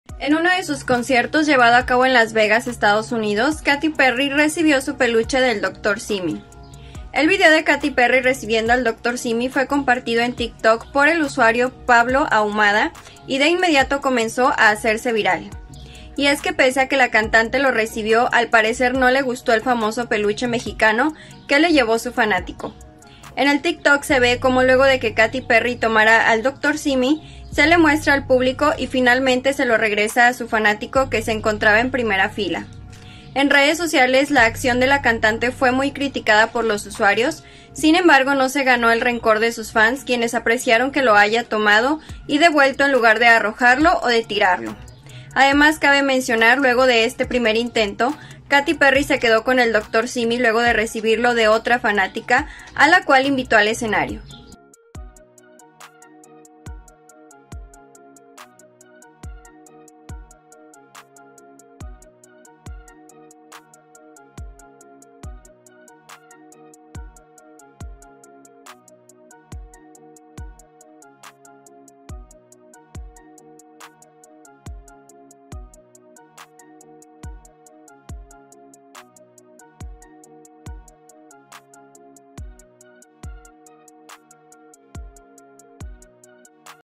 Katy Perry ya recibió su Doctor Simi, pero no le gustó
Durante un concierto en Las Vegas, Estados Unidos, la cantante Katy Perry recibió su Doctor Simi.

Pero el famoso peluche mexicano no le gustó a la cantante estadounidense de 38 años de edad.

*** Descarga la App Mercado Pago y obtener $100 🎁 de descuento en tu primer pago